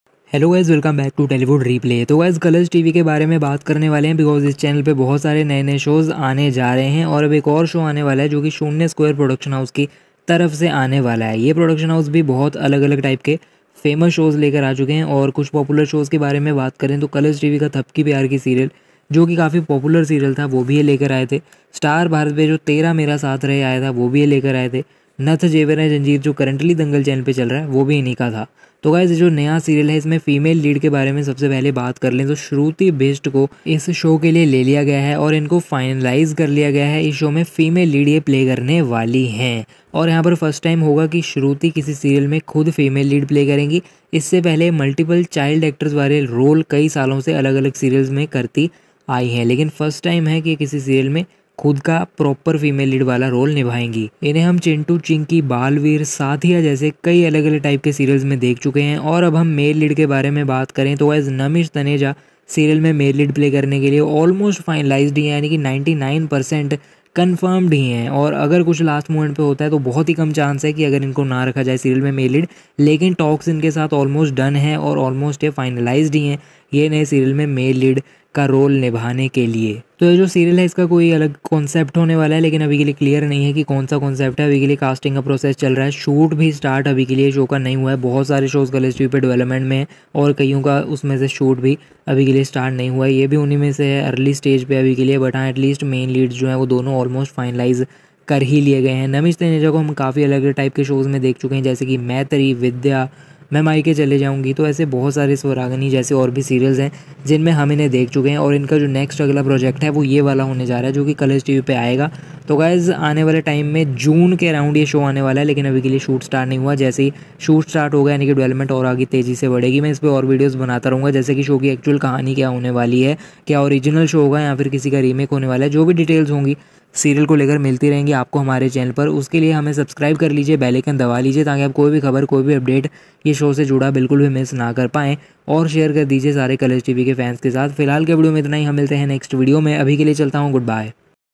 Namish Taneja & Shruti Bisht to play LEADS in Colors TV New Show | Upcoming Serial Details
About the video - I talk about Colors TV’s new upcoming show by Shoonya Square Productions and its newly cast lead actors Namish Taneja and Shruti Bhist.